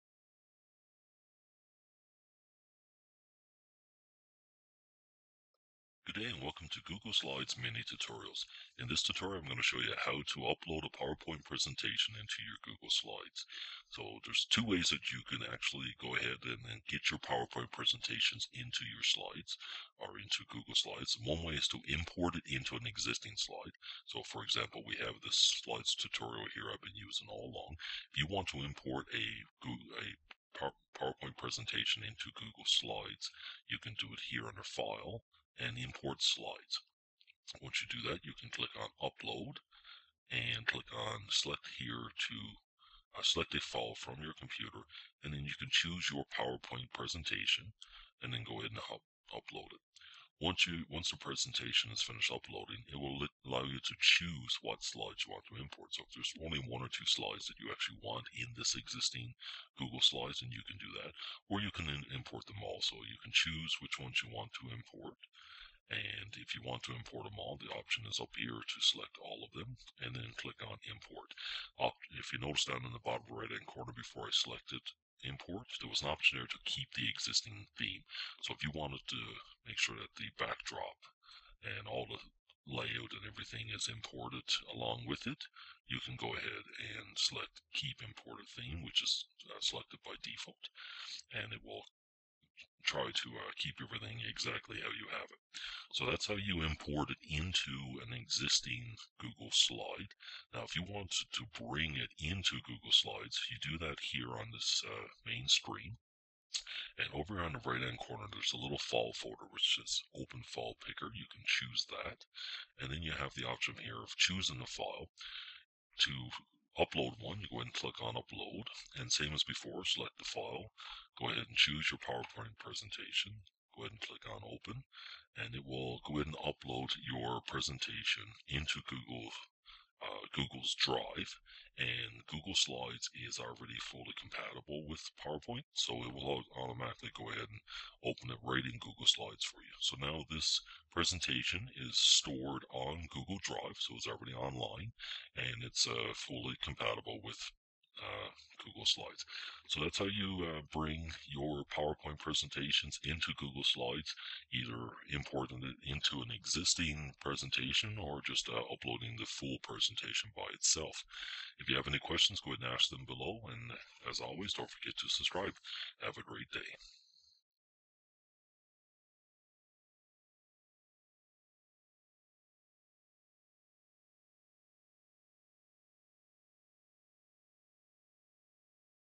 How to Upload Powerpoint to Google Slides
A mini Google Slides tutorial on how to upload powerpoint to google slides

Related:
upload powerpoint to google slides
powerpoint online google
google powerpoint presentation
powerpoint google drive